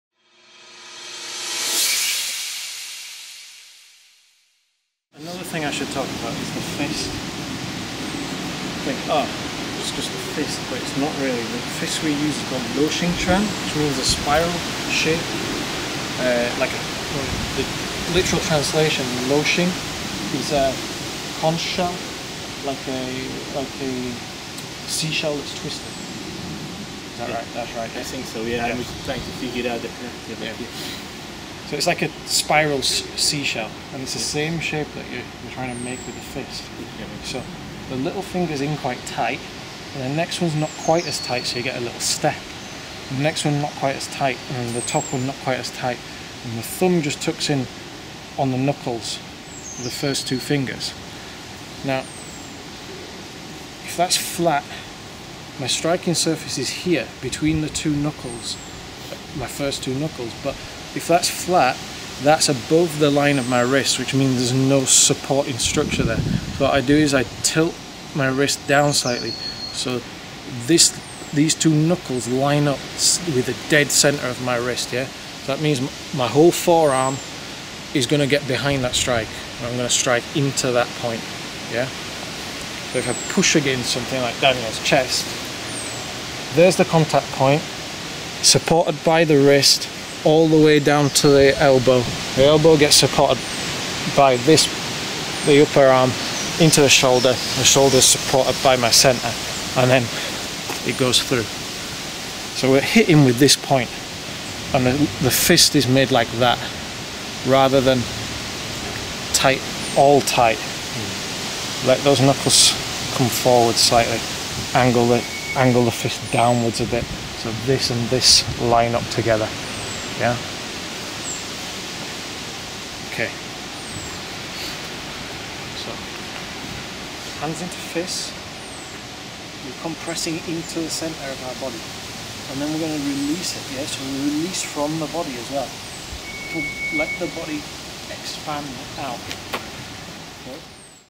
Xing Yi Quan Fist: Conch shell fist! Luoxingquan [Xingyiquan / Hsing I Chuan]
It takes time, effort and money to make content and we greatly appreciate any support through small donations.

Paul Andrews takes time out from teaching Beng Quan to demonstrate the subtleties of the conch shell fist.

This fist can deliver devastating power when used with all your structure behind it and is the standard first fist we learn in Xing Yi Quan. It originated from how we traditionally hold a sword.

Remember you can get a FREE 4 Part Mini Xing Yi Course right